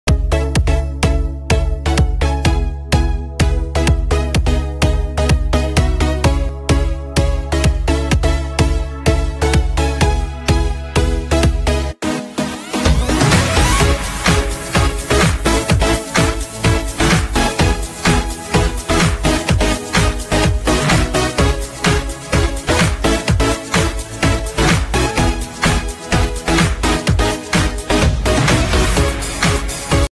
挑战自制“花样手机壳”，印出来的花色到底有多好看？
挑战自制“花样手机壳”，印出来的花色到底有多好看？,本视频由最强折腾王提供,4289次播放,好看视频是由百度团队打造的集内涵和颜值于一身的专业短视频聚合平台